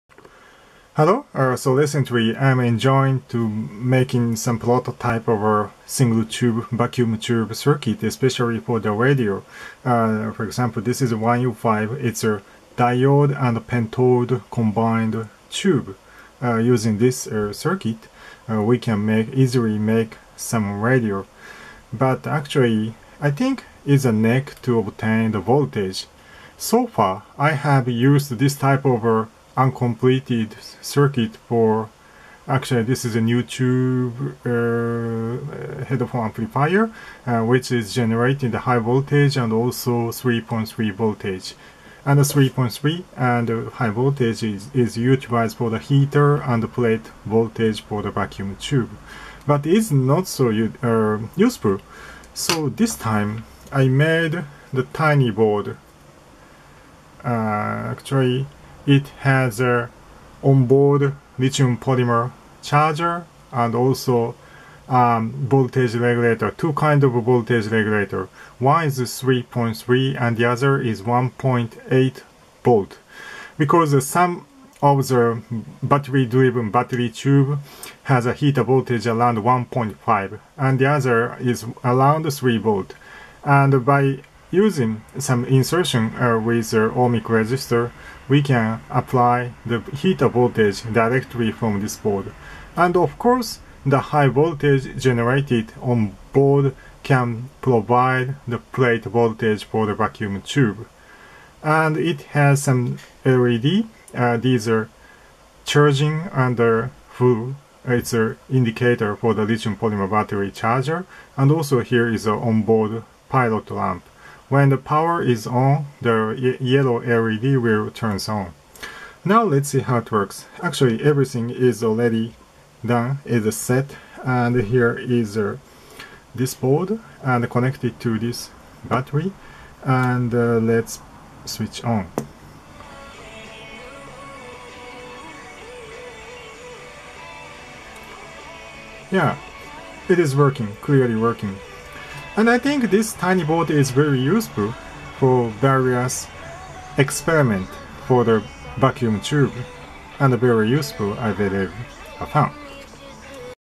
All-in-one power supply for vacuum tube circuit experiment driven by Lipo
Recently I am enjoying vacuum tube circuit experiment on breadboard. I guess the neck for this joy is "power supply". All of vacuum tube requires heater and plate voltage and heater should be high around or more than 30V even battery-driven vacuum tube. This time I made an all-in-one power supply consisting of Lipo charger, 1.8V LDO and 3.3 V LDO and voltage booster generating 30V. Yeah, just this board is enough to explore vacuum tube!